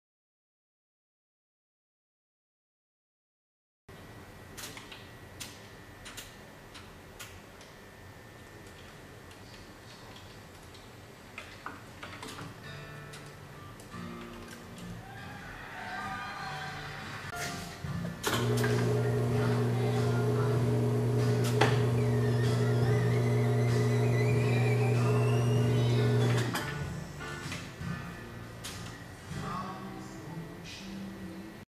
Produce_1.mpg
Giant robotic hand.  This is a 9-foot tall robotic hand designed for science museums and technical  trade shows.  It is 90% complete to the true biomechanics of a human hand at 10X scale.  We discovered that the first thing people want to do is flip the bird, so why not make that our first vid!